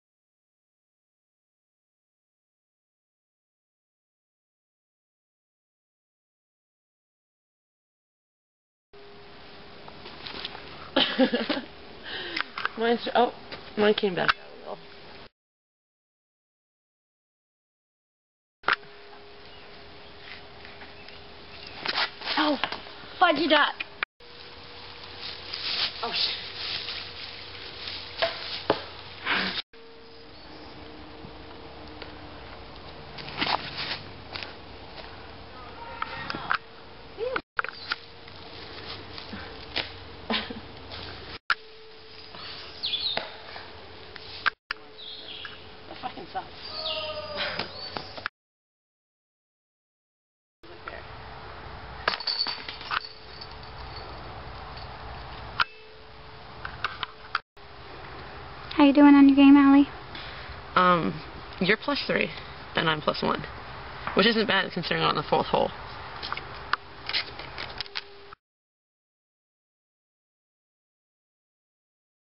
2 Chicks Playing Disc Golf
Me and Ali love disc golf...so it's no surprise I took my camera with us when we went to Enman Field in Brunswick, ME.